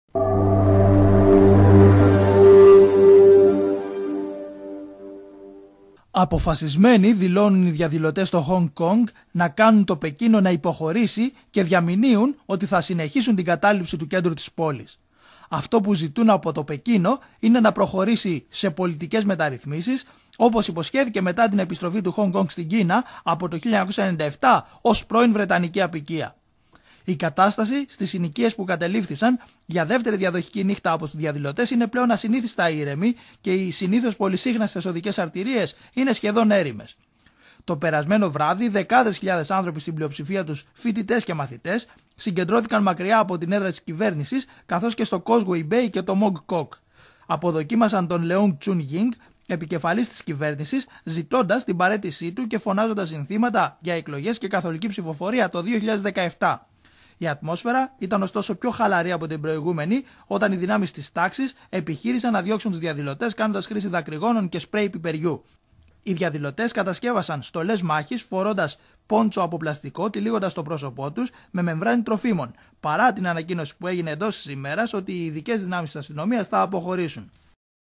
Αποφασισμένοι δηλώνουν οι διαδηλωτές στο Χονγκ Κονγκ
Οι διαδηλωτές στο Χονγκ Κονγκ δηλώνουν ότι θα συνεχίσουν την κατάληψη του κέντρου της πόλης μέχρι το Πεκίνο να υποχωρήσει στις πολιτικές μεταρρυθμίσεις που τους είχαν υποσχεθεί όπως υποστηρίζουν,μετά την επιστροφή το 1997,στην Κίνα από πρώην βρετανική αποικία.